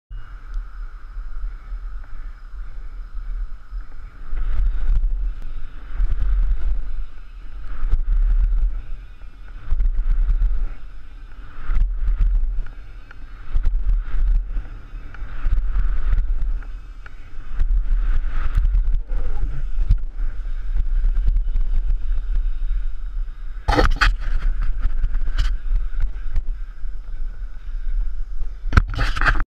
gopro backflip bmx
It's 97 degrees out and I'm not used to this heavy, hot ass helmet for bmx.
not to mention its pointing way to far down. only showing the bike.